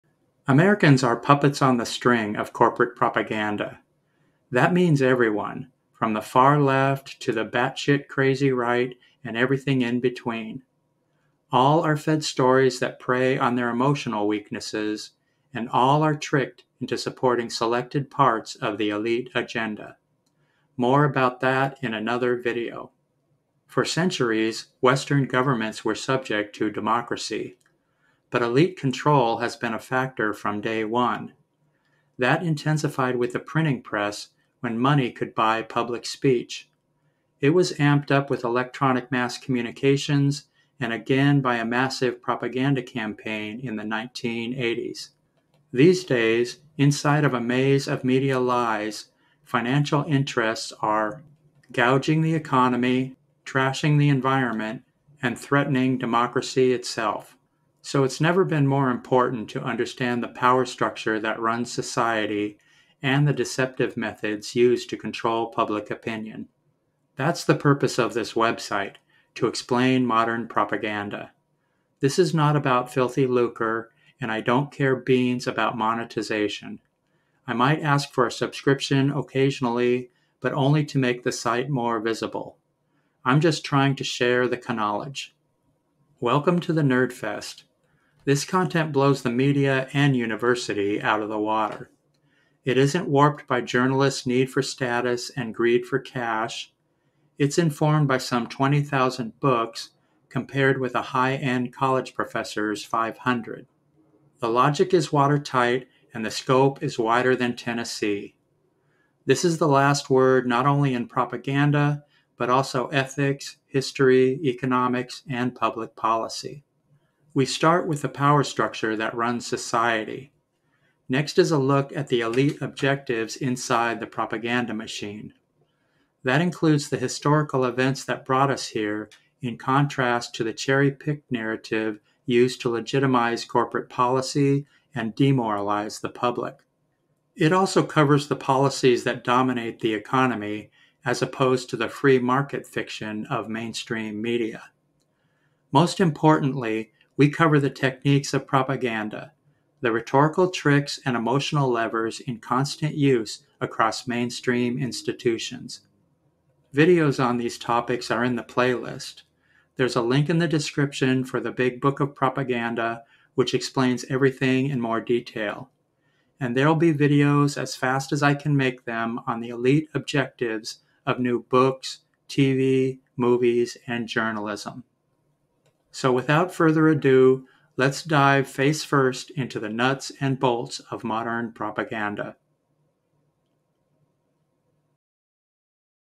Intro to Propaganda
An overview of the website. Topics covered include the power structure that runs society, the policies that dominate the economy, and most importantly, the propaganda methods used across mainstream institutions.

0:00 Intro to Propaganda
1:06 Website Purpose